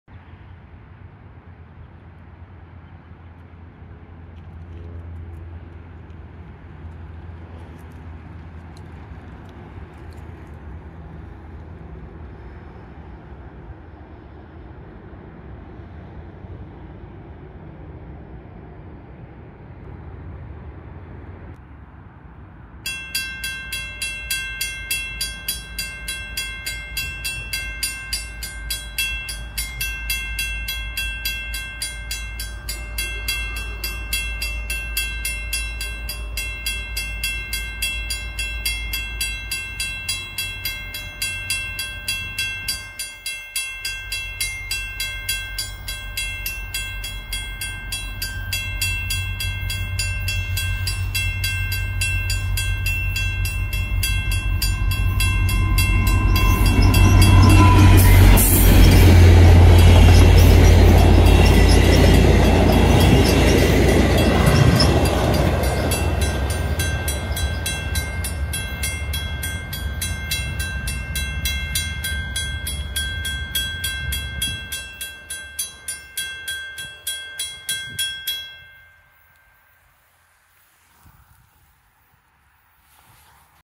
Train 53 leaving Ottawa at the Brookfield Pathway (Beachburg Sub mi 3.57)
Seeing my dad off, pic at the end is his.
Also FLIRT jumpscare.